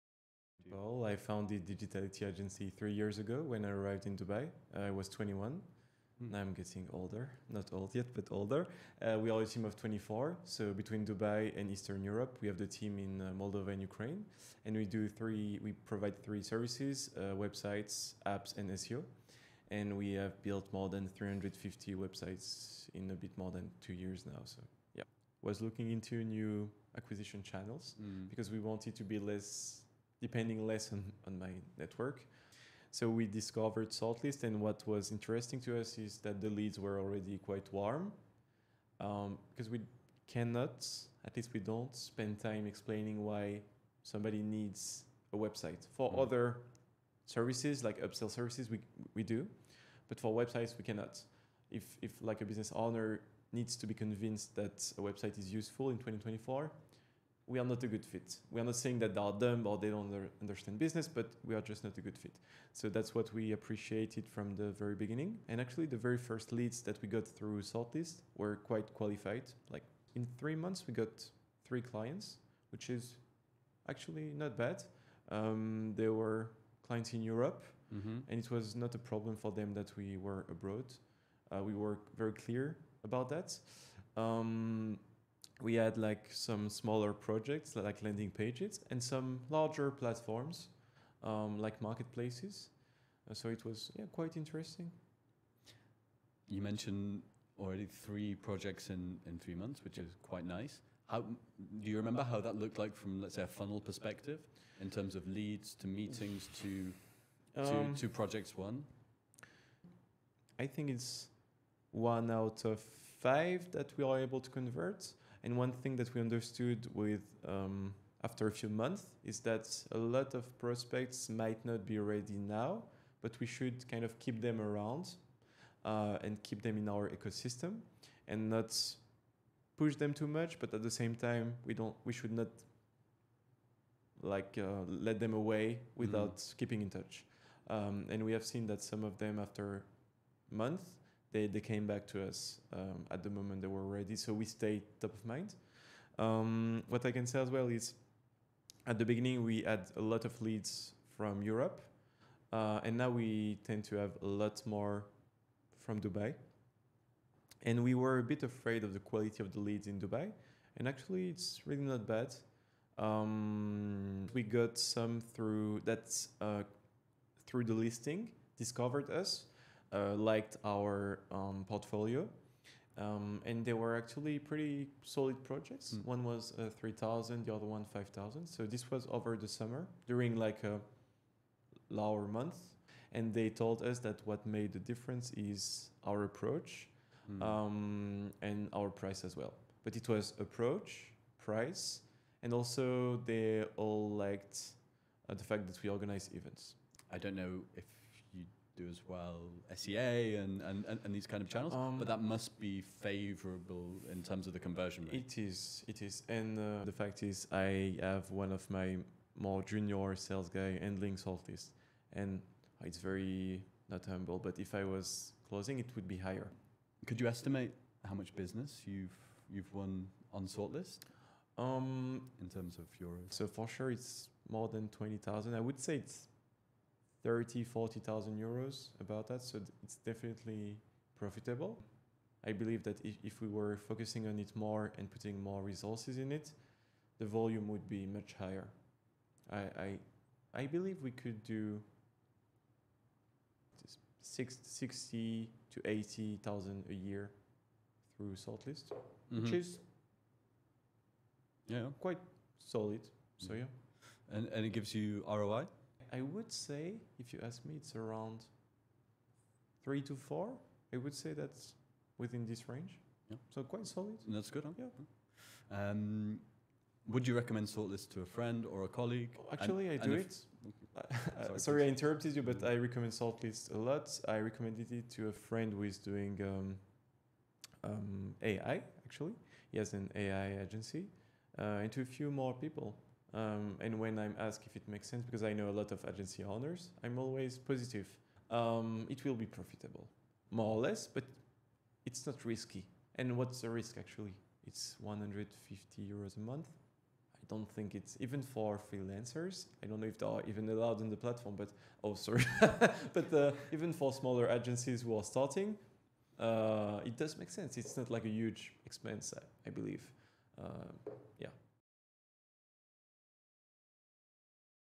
"3X or 4X ROI with Sortlist"
Paul Darmas from Dubai based web agency Digitality, tells us about his experience with Sortlist.

If you're running an agency, and are interested in the same,